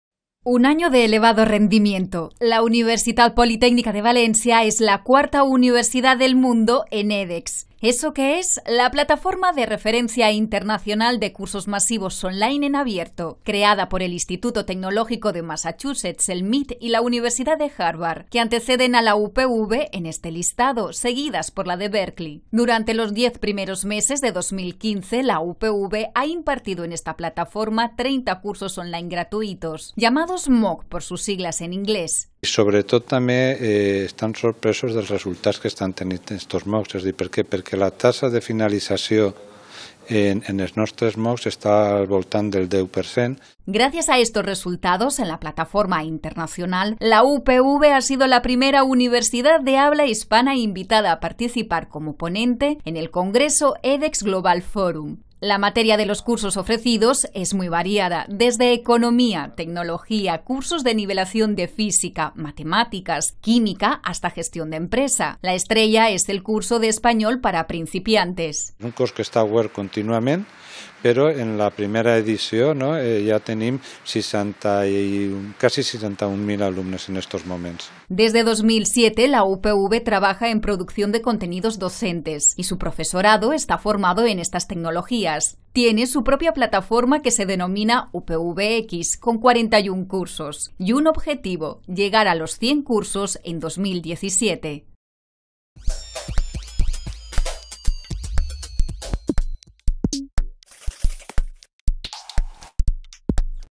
Noticia @UPVTV: UPV, cuarta en @edXOnline [20-11-2015]
Un año de elevado rendimiento. La Universitat Politécnica de Valencia es la cuarta universidad del mundo en edX. ¿Eso qué es? La plataforma de referencia internacional de cursos masivos on-line, en abierto. Creada por el Instituto Tecnológico de Massachussets, el MIT y la Universidad de Harvard, que anteceden a la UPV en este listado, seguidas por la de Berkeley.

Durante los 10 primeros meses de 2015 la UPV ha impartido en esta plataforma, 30 cursos on line gratuitos, llamados MOOC por sus siglas en inglés.

Gracias a estos resultados en la plataforma internacional, la UPV ha sido la primera universidad de habla hispana invitada a participar, como ponente, en el congreso edX Global Forum.
La materia de los cursos ofrecidos son muy variadas, desde economía, tecnología, cursos de nivelación de física, matemáticas, física, química hasta gestión de empresa. La estrella es el curso de español para principiantes.

Desde 2007 la UPV trabaja en producción de contenidos docentes y su profesorado está formado en estas tecnologías. Tiene su propia plataforma que se denomina UPVx, con 41 cursos.
Y un objetivo: llegar a los 100 cursos en 2017.